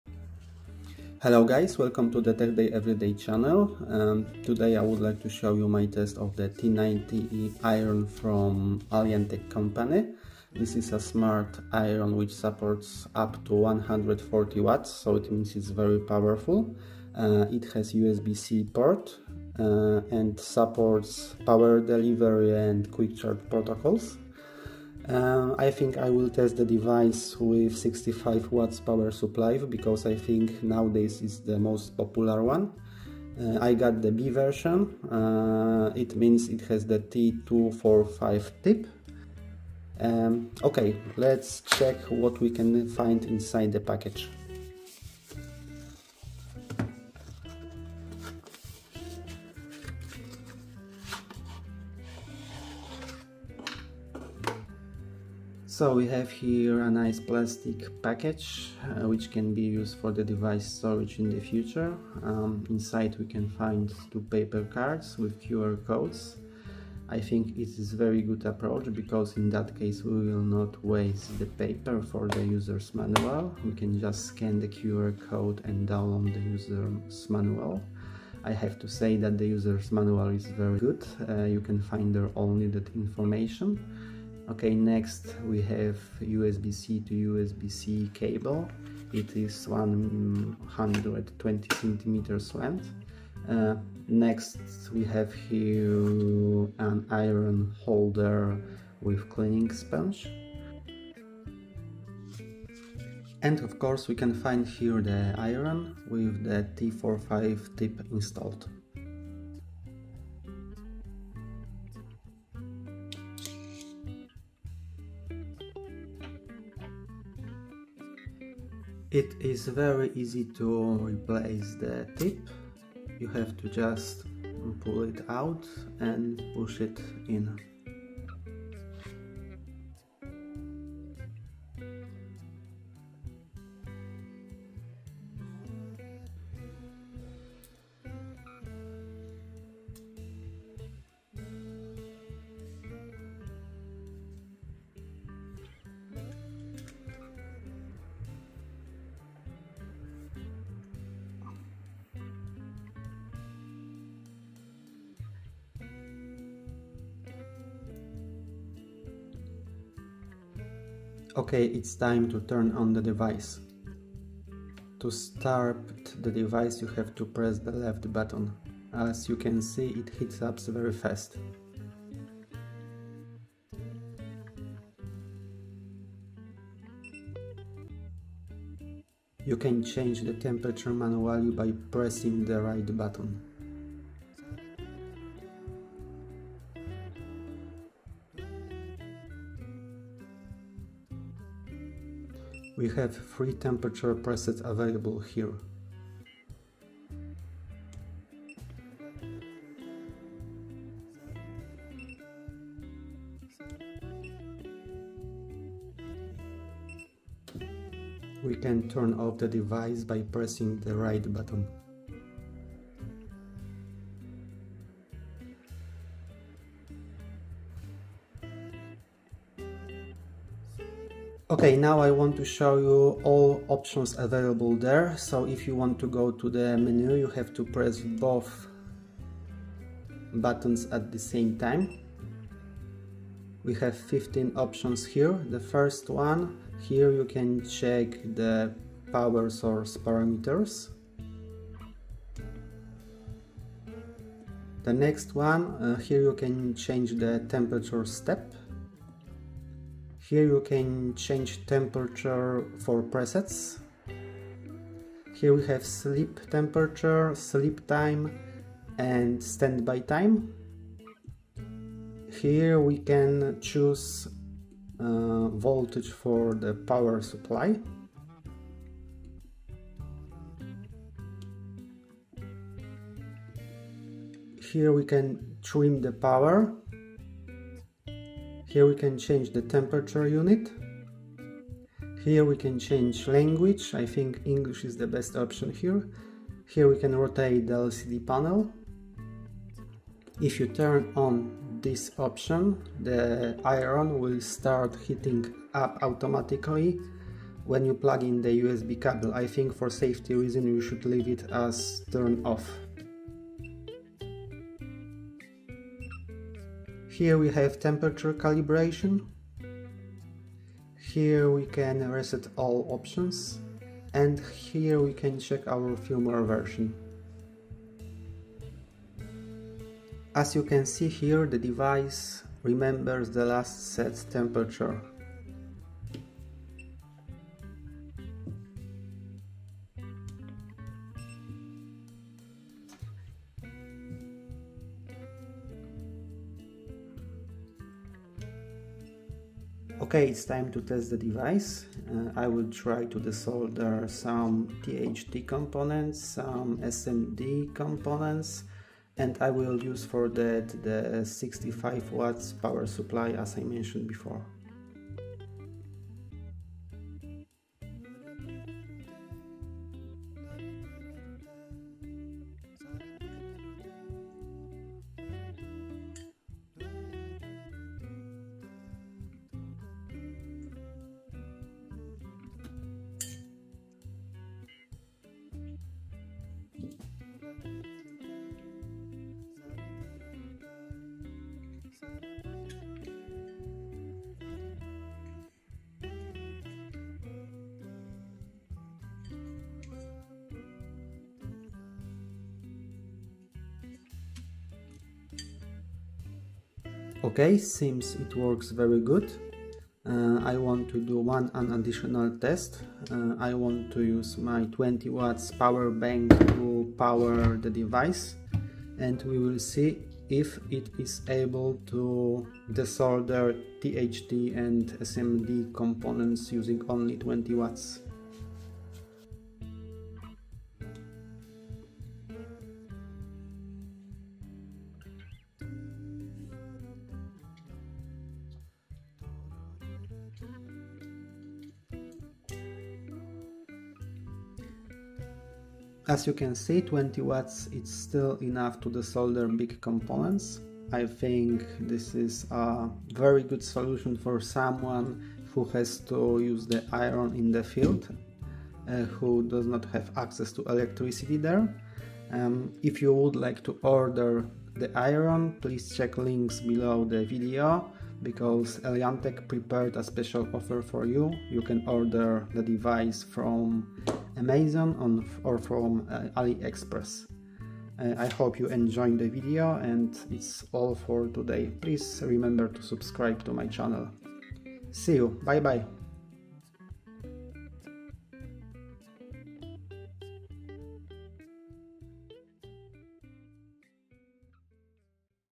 ALIENTEK T90 USB-C: 140W Soldering Iron with QC/PD! | UNBOXING, TEST & FULL REVIEW
USB-C powered soldering irons are the future! Can the Alientek T90 revolutionize the repair bench and prove to be the best portable soldering iron?

In this comprehensive review, I unbox and thoroughly test the Alientek T90, which can deliver a powerful **140W**!

🔥 **KEY FEATURES:**
* **Power:** Up to 140W (when using an appropriate PD/QC power adapter).
* **Power Source:** Universal USB-C port.
* **Protocols:** Supports **Power Delivery (PD)** and **Quick Charge (QC)**
* **Model:** Alientek T90B.

**VIDEO TIMESTAMPS:**
0:00 Unboxing and First Impressions
2:40 Heating Speed Test
3:40 Available options
5:38 Soldering test  (65W)
6:36 Soldering test (20W)
7:15 Summary and Final Verdict